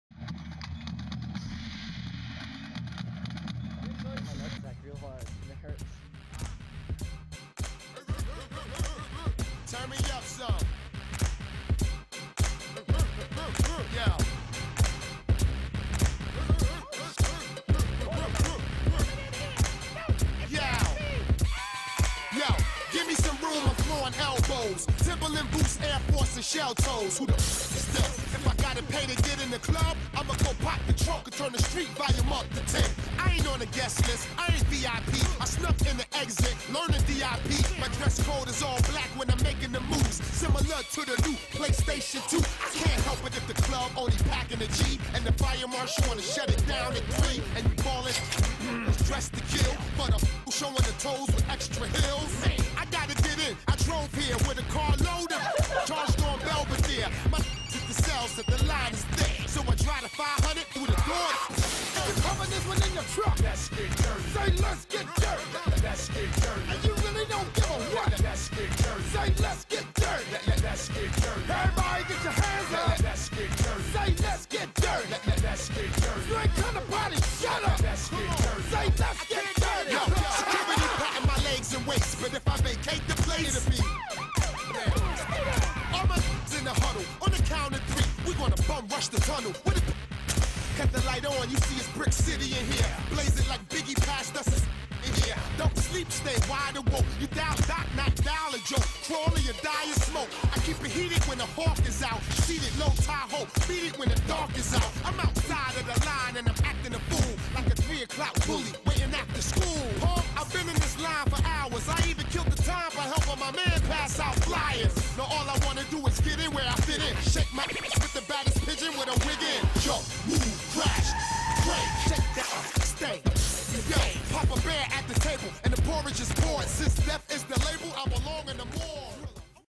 This is the Organization XIII way!!
k so another super fun night with my homies! taught azn2luv how to handspring and it was awesome! made this video and was FUN! and i did poop while everyone else did good. soooo yea enjoy SUBSCRIBE LIKE COMMENT TIPS need all the help people!